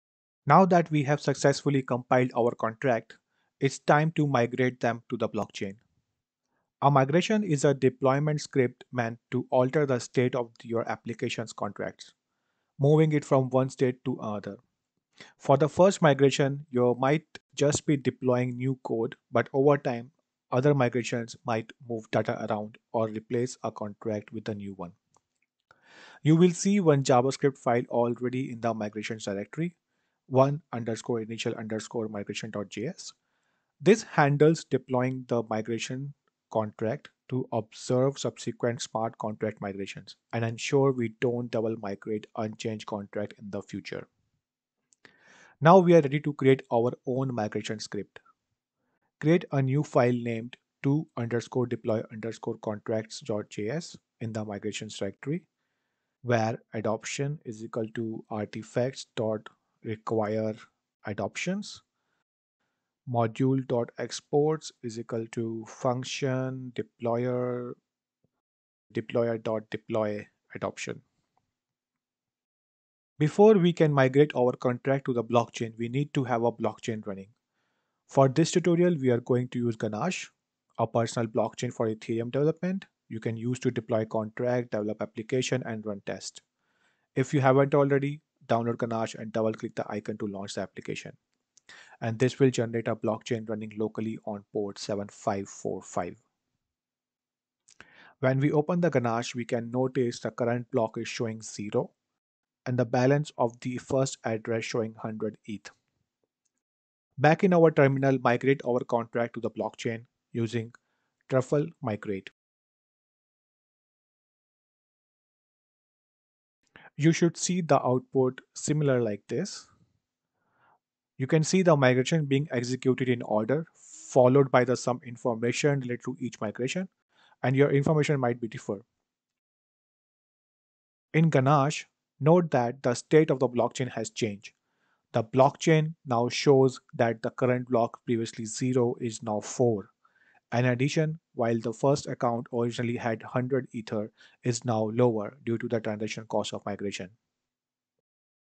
Lesson 6 - Migration [Create your First DApp using Truffle and Ganache]
A migration is a deployment script meant to alter the state of your application's contracts, moving it from one state to the next. For the first migration, you might just be deploying new code, but over time, other migrations might move data around or replace a contract with a new one.

You'll see one JavaScript file already in the migrations/ directory: 1_initial_migration.js. This handles deploying the Migrations.sol contract to observe subsequent smart contract migrations, and ensures we don't double-migrate unchanged contracts in the future.

Now we are ready to create our own migration script. Create a new file named 2_deploy_contracts.js in the migrations/ directory.

Before we can migrate our contract to the blockchain, we need to have a blockchain running. For this tutorial, we're going to use Ganache, a personal blockchain for Ethereum development you can use to deploy contracts, develop applications, and run tests. If you haven't already, download Ganache and double click the icon to launch the application. This will generate a blockchain running locally on port 7545.

Back in our terminal, migrate the contract to the blockchain.

truffle migrate

You should see output similar to the following:You can see the migrations being executed in order, followed by some information related to each migration. (Your information will differ.)

In Ganache, note that the state of the blockchain has changed. The blockchain now shows that the current block, previously 0, is now 4. In addition, while the first account originally had 100 ether, it is now lower, due to the transaction costs of migration. We'll talk more about transaction costs later.

You've now written your first smart contract and deployed it to a locally running blockchain. It's time to interact with our smart contract now to make sure it does what we want.